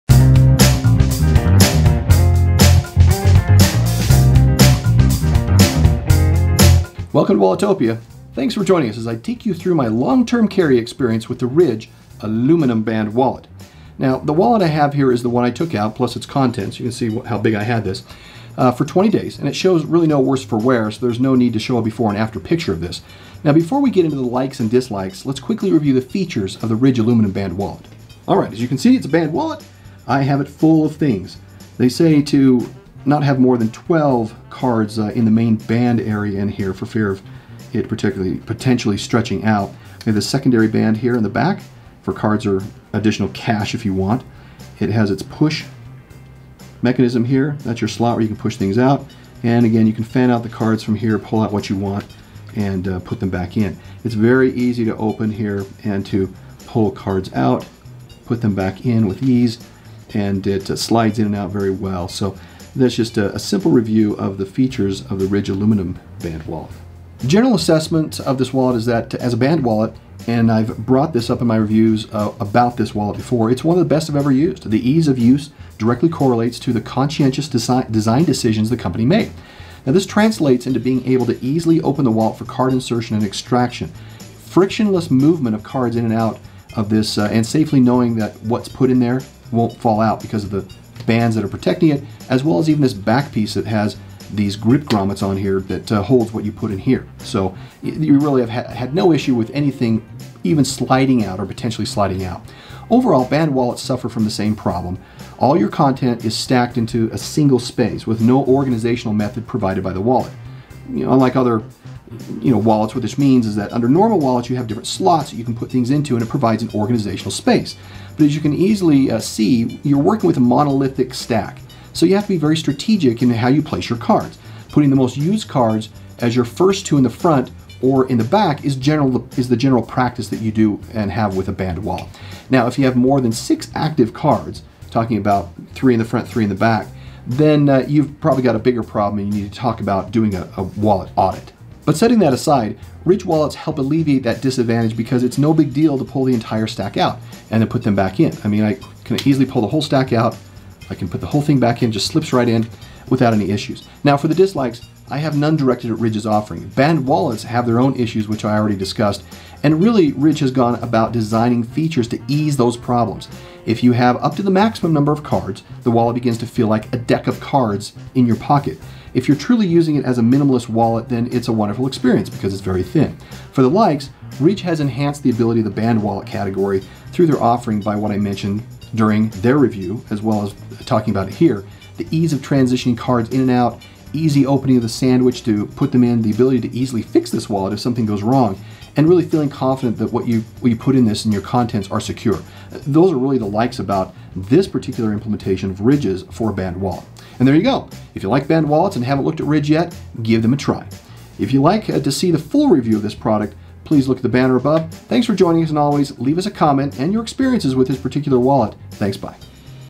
Ridge’s Aluminum band wallet was flawless…for a band wallet
There are inherent design problems with the band wallet category; monolithic stack of cards to deal with, difficulty in organizing and pulling out exactly what you need and what can be a hard time working the deck of cards in and out.  But Ridge’s design and specifically the aluminum band we tested proved not only does it function at the highest levels for a band wallet, but their design is specifically helps address short-comings in band wallets. @RidgeWalletYT

Ridge Aluminum band wallet
Citizen Eco-Drive Silvertone & Black World Time
Pelikan M600 Green /Black GT

TIMESTAMPS:
Intro - 0:00
Unboxing - 00:21
Feature Review - 00:28
Measurement & Weight - 01:04
Card & Cash Insertion Test - 02:04
Review Summary/Detail - 02:31
Pocket Insertion Test - -
Pricing - -
Final Score - 03:07

What wallets should be in a long-term carry test?
What is the most important features of a wallet?